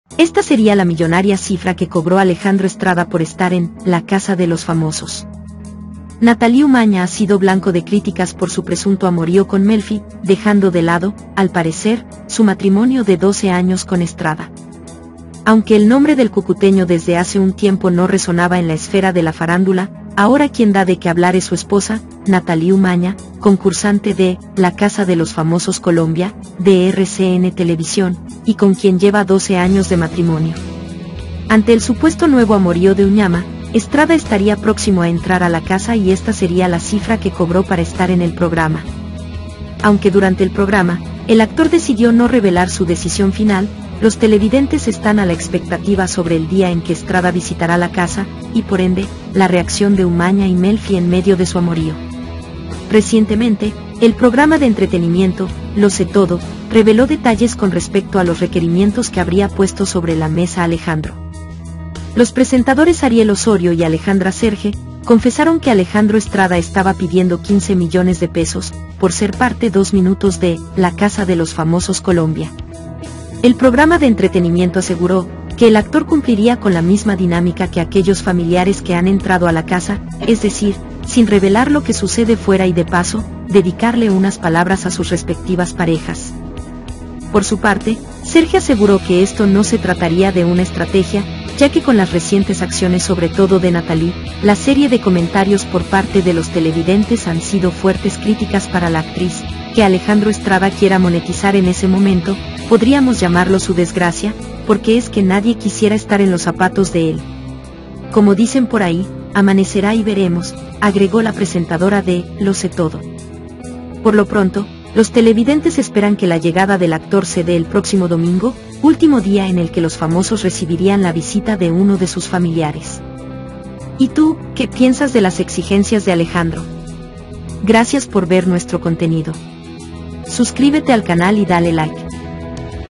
💥Atención! ALEJANDRO HIZO MILLONARIA EXIGENCIA PARA ENTRAR A "La casa de los famosos"! YA HAY FECHA!
Ante el supuesto nuevo amorío de Nataly  Umaña, Estrada estaría próximo a entrar a la casa y esta sería la cifra que cobró .